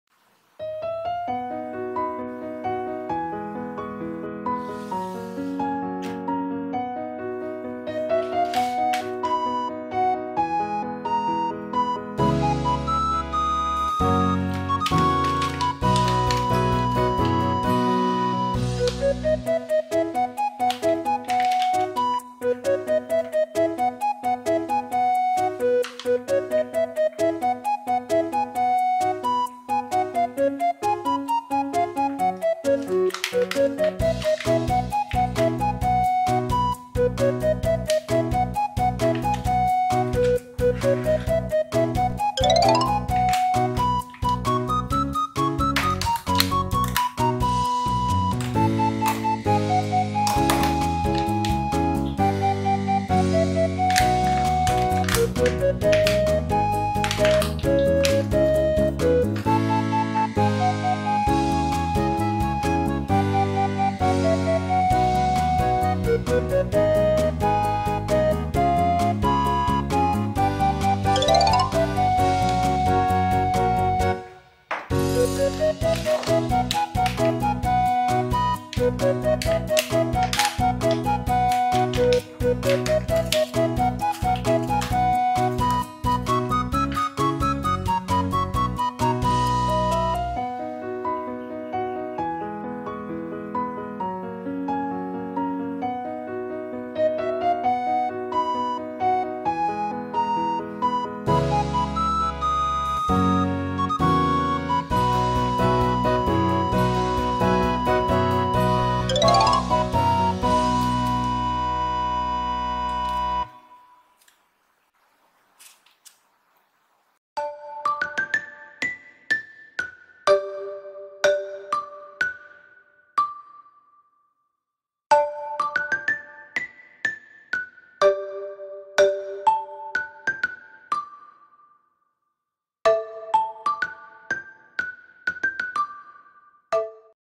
Crayon Shin Chan Gummy Candies - Chocolate & Yogurt 【 GiftWhat 】
This is crayon shin chan gummy candies. This candies have chocolate and yogurt flavors. It's shinchan candy from Korea. Inside have surprise shin-chan sticker. Each pack only have one sticker.